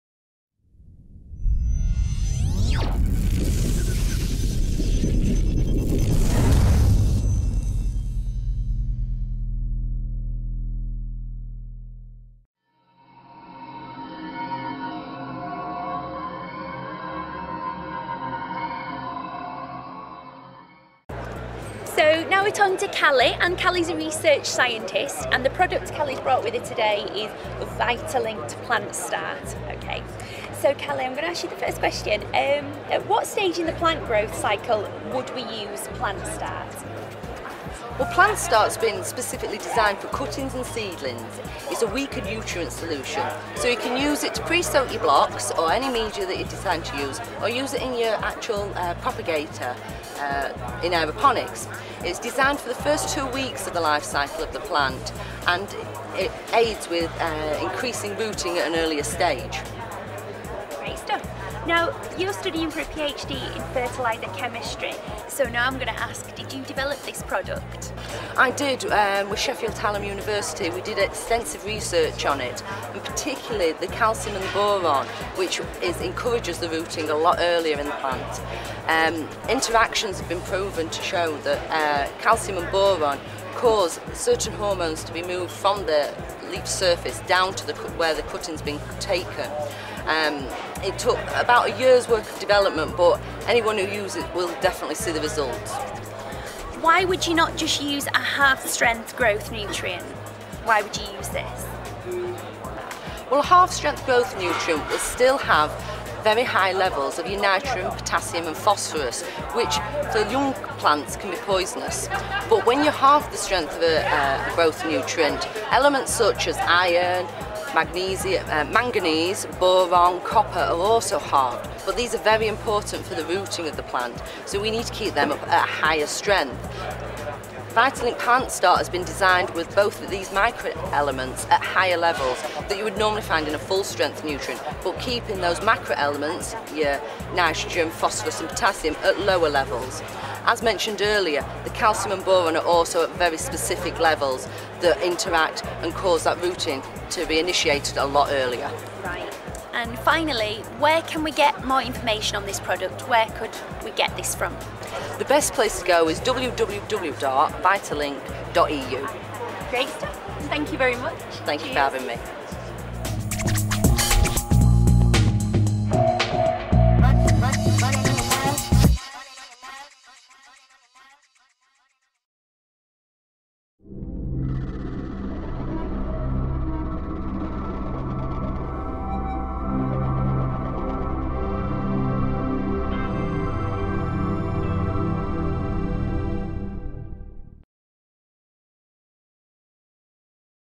VitaLink PlantStart @ Grow Expos 2012 HYDROSHOW.tv
Callie from VitaLink talks us through a product she developed herself with the assistance of Sheffield Hallam University. The Plant Start product is designed as a replacement for often used half strength nutrients, with specific minerals for seedling stage of the plants life.